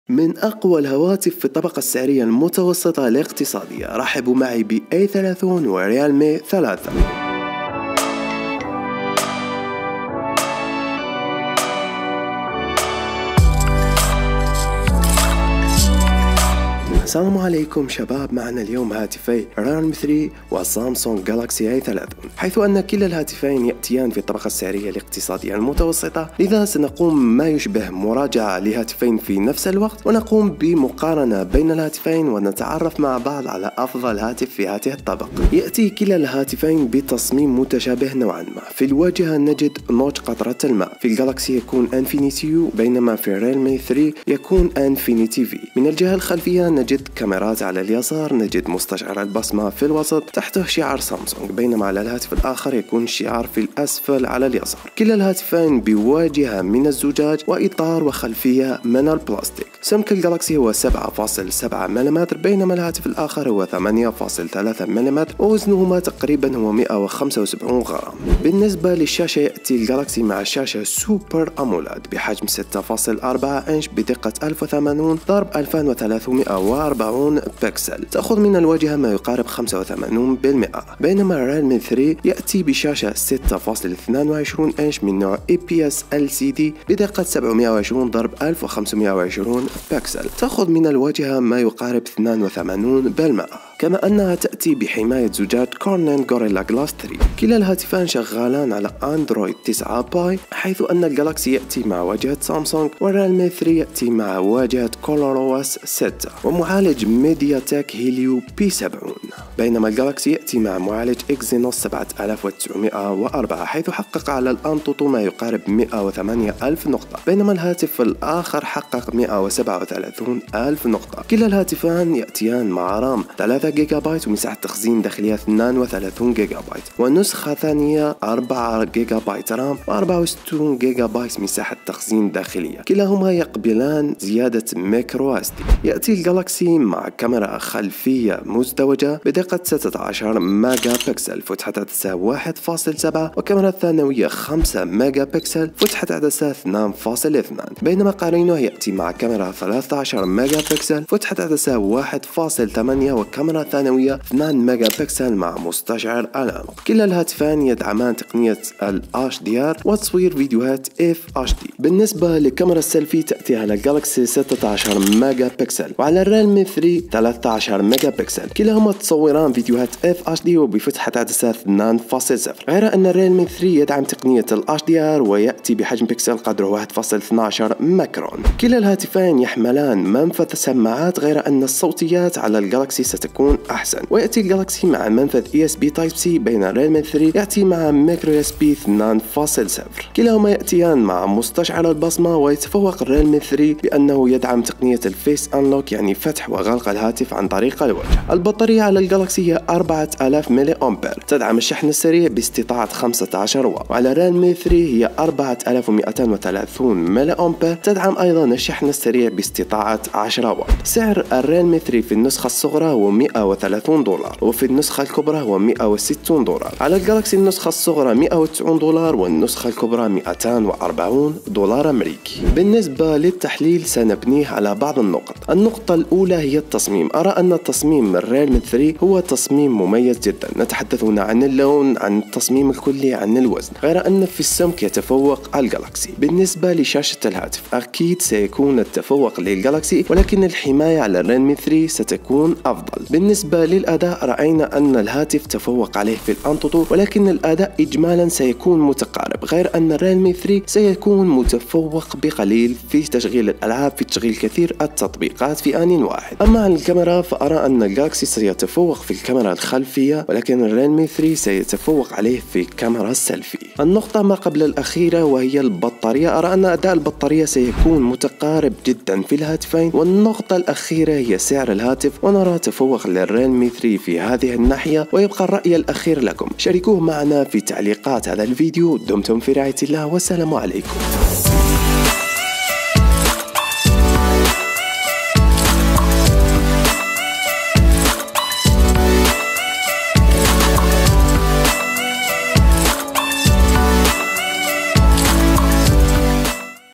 رسميا سعر و مواصفات Samsung Galaxy A30 | هل يستحق الشراء 🔥
📣📢📣  إشترك في قناتنا يا فخم 📣📢📣

شاهد المقطع كاملا رحمك الله يا عبد
لا تقصر معي بنشر الفيديو حبيبي في الله